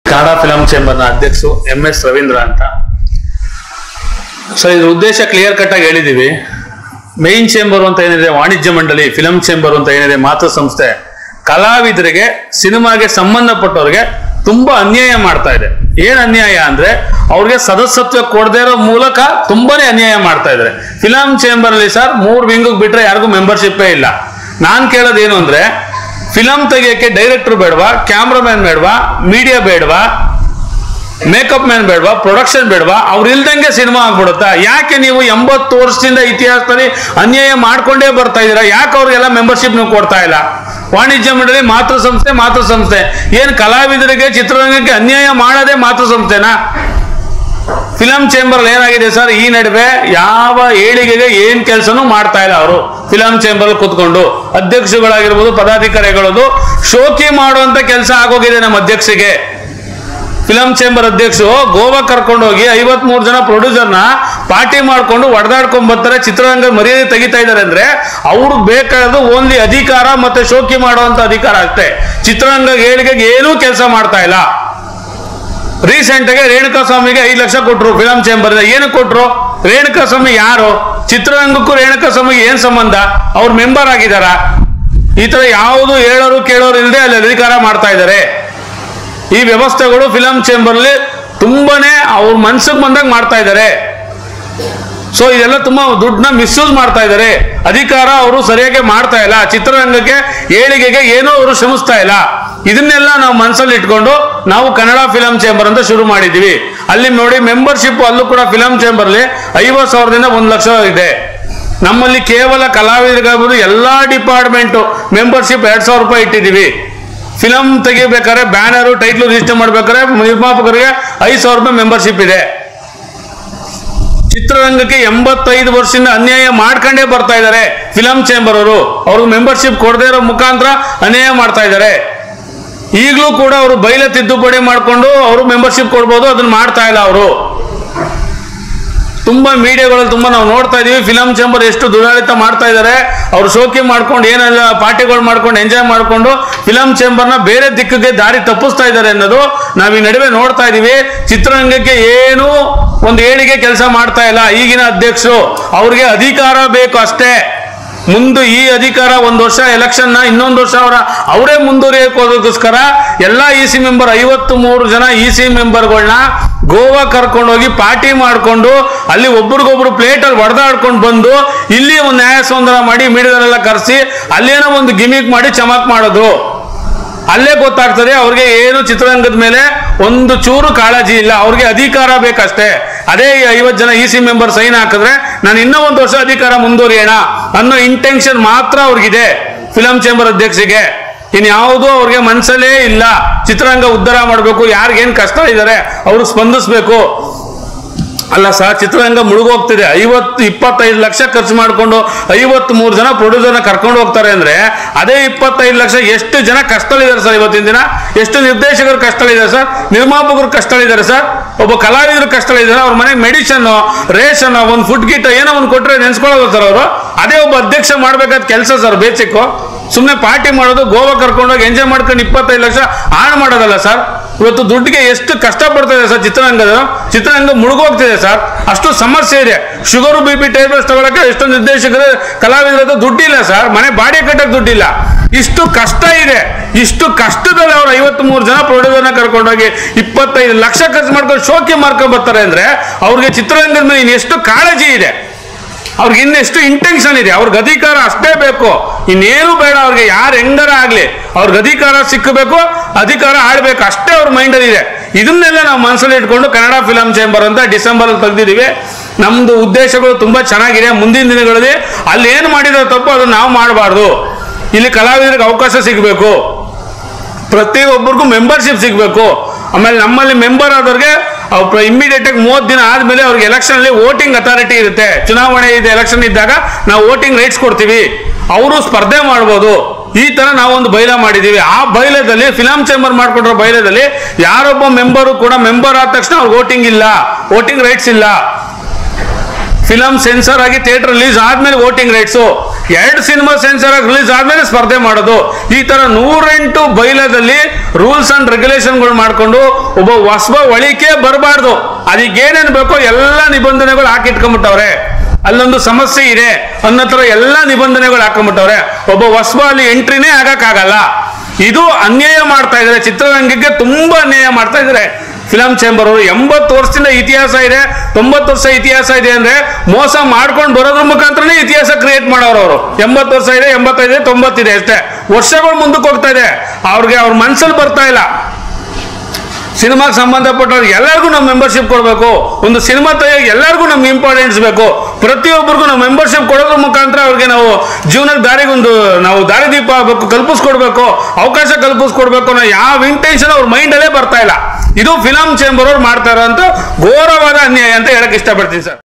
ಹೊಸದಾಗಿ ಪ್ರಾರಂಭವಾದ ಕನ್ನಡ ಫಿಲಂ ಚೇಂಬರ್ ಪೋಸ್ಟರ್ ಬಿಡುಗಡೆ.INDUNEWS6#
ಹೊಸದಾಗಿ ಪ್ರಾರಂಭವಾದ ಕನ್ನಡ ಫಿಲಂ ಚೇಂಬರ್ ಪೋಸ್ಟರ್ ಬಿಡುಗಡೆ.
ಕನ್ನಡ ಫಿಲಂ ಚೇಂಬರ್ ಸಿನಿಮಾಗೆ ಸಂಭಂದಪಟ್ಟ ಕಲಾವಿದರು ಸದಸ್ಯವ್ವ 2000
ನಿರ್ಮಾಪಕರು ಮೂವಿ  ಬ್ಯಾನರ್ ಲೈಫ್ ಟೈಮ್ ಸದಸ್ಯವ್ವ5000 ಎಂ.ಎಸ್.ರವೀಂದ್ರ ಅಧ್ಯಕ್ಷರು